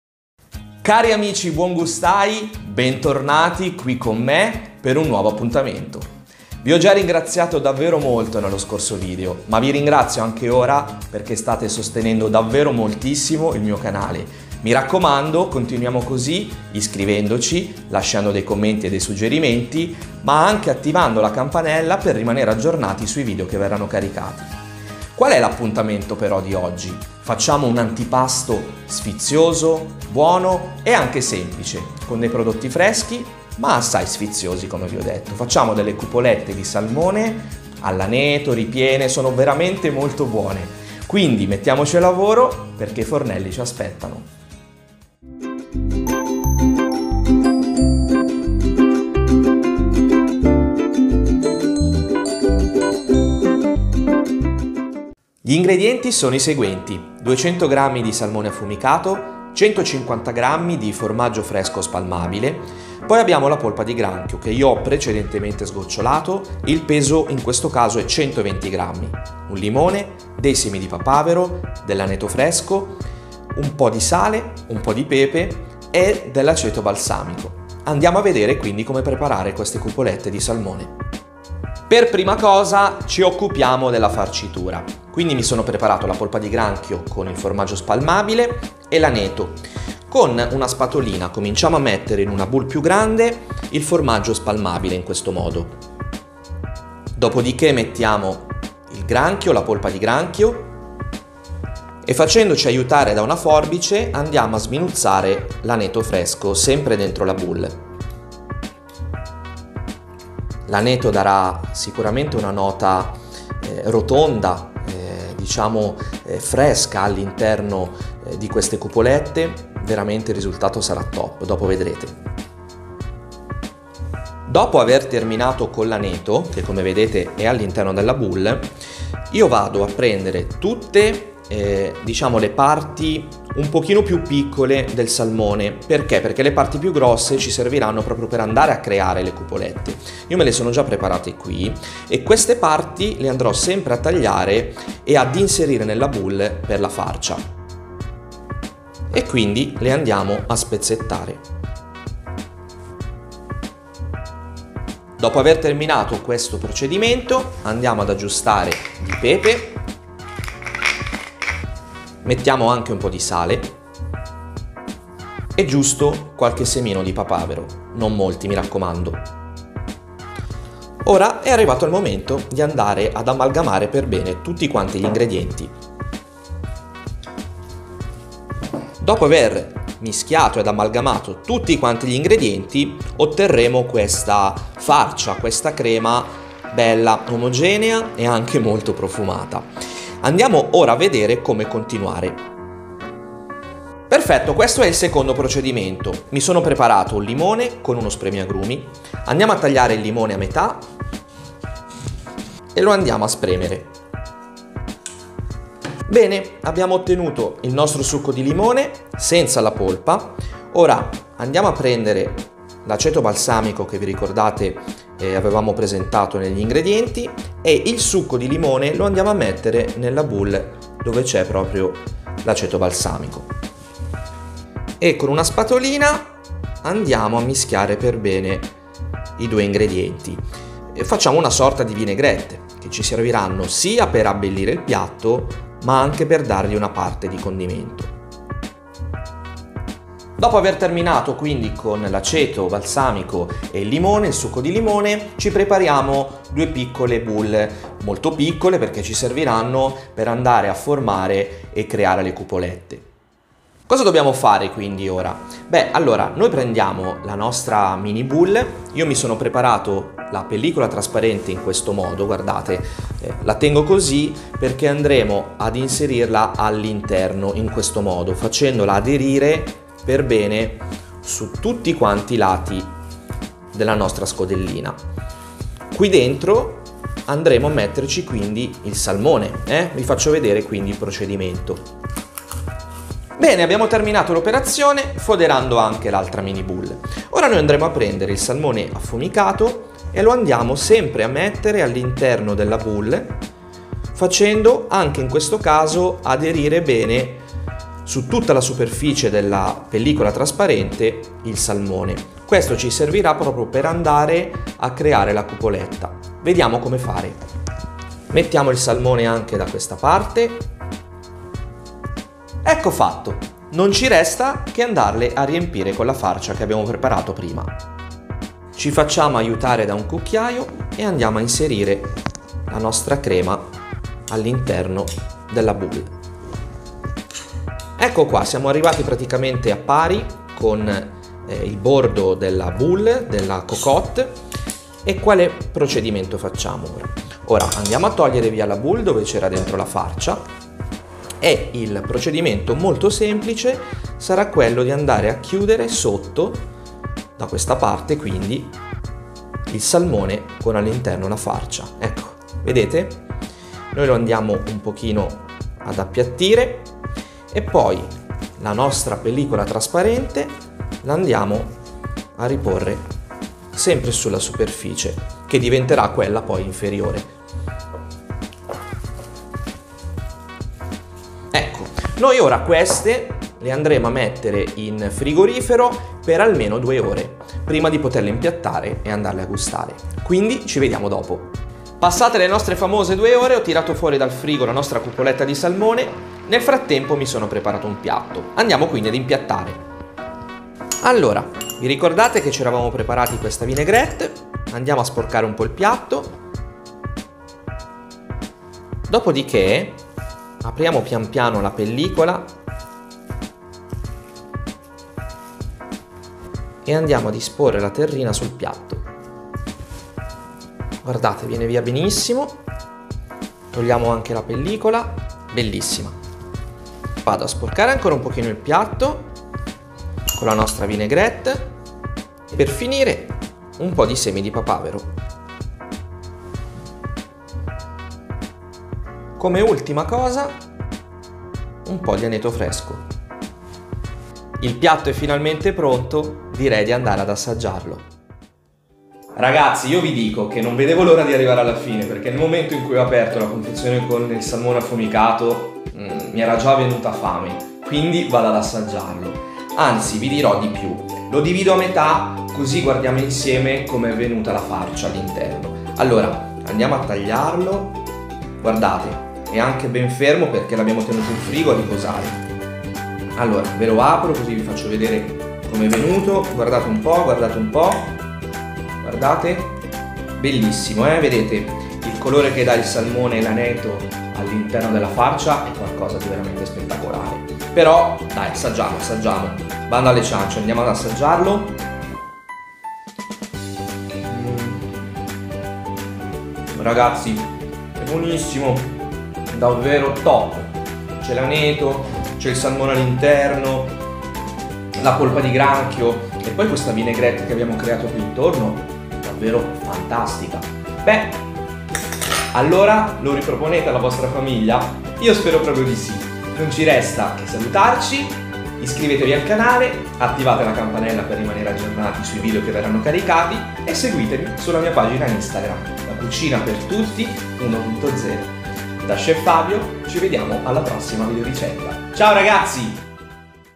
Cupolette di salmone all'aneto - (ANTIPASTO PERFETTO)
Ingredienti per due/tre cupolette:
200 gr. di salmone affumicato
120 gr. di polpa di granchio
150 gr. di formaggio fresco spalmabile
1 limone
Un goccio di aceto balsamico
Semi di papavero q.b.
Aneto fresco
Pepe q.b
Sale q.b

Buon appetito😁